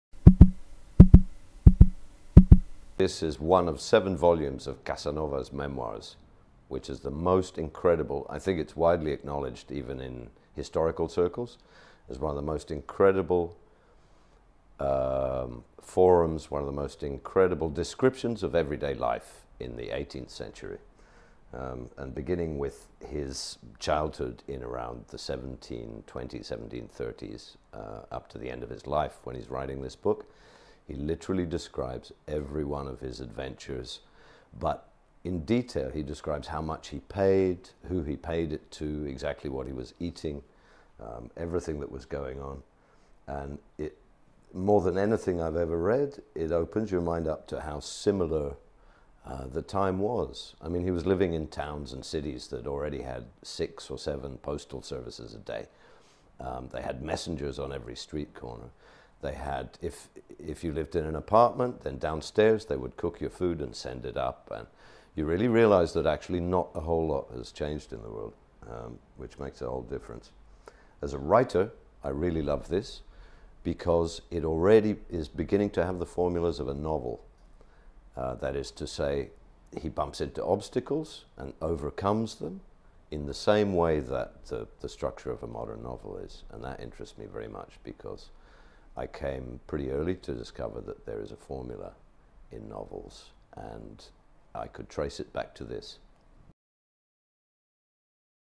DBC Pierre over The Memoirs of Casanova
– BOEKENTIPS IN 1 MINUUT – "It is one of the most incredible descriptions of everyday life in the eighteenth century." – DBC Pierre, 53, schrijver, Londen.